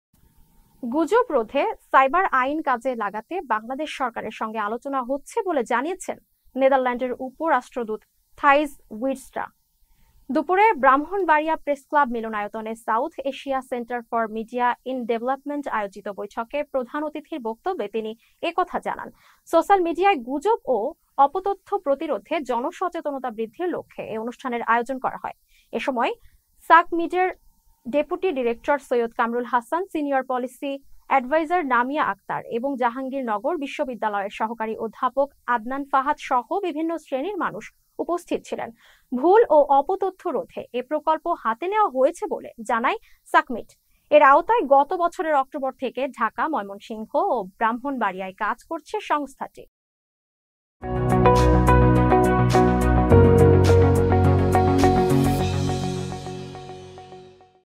`গুজব রোধে সাইবার আইন নিয়ে বাংলাদেশ সরকারের সঙ্গে আলোচনা হচ্ছে' | Rumor Awareness | Channel S News
গুজব রোধে সাইবার আইন কাজে লাগাতে বাংলাদেশ সরকারের সঙ্গে আলোচনা হচ্ছে বলে জানিয়েছেন নেদারল্যান্ডের উপ-রাষ্ট্রদূত থাইজ উইডস্ট্রা। দুপুরে ব্রাহ্মণবাড়িয়া প্রেসক্লাব মিলনায়তনে সাউথ এশিয়া সেন্টার ফর মিডিয়া ইন ডেভেলপমেন্ট আয়োজিত বৈঠকে প্রধান অতিথির বক্তব্যে তিনি এ কথা জানান। সোশ্যাল মিডিয়ায় গুজব ও অপতথ্য প্রতিরোধে জনসচেতনতা বৃদ্ধির লক্ষ্যে এ অনুষ্ঠানের আয়োজন করা হয়। এ সময় সাকমিডের ডেপুটি ডিরেক্টর সৈয়দ কামরুল হাসান, সিনিয়র পলিসি এডভাইজার নামিয়া আক্তার এবং জাহাঙ্গীরনগর বিশ^বিদ্যালয়ের সহকারি অধ্যাপক আদনান ফাহাদসহ বিভিন্ন শ্রেণির মানুষ উপস্থিত ছিলেন। ভুল ও অপতথ্য রোধে এ প্রকল্প হাতে নেয়া হয়েছে বলে জানায় সাকমিড। এর আওতায় গত বছরের অক্টোবর থেকে ঢাকা, ময়মনসিংহ ও ব্রাহ্মণবাড়িয়ায় কাজ করছে সংস্থাটি।

1: News :
2: Entertainment News :
3: Sports News :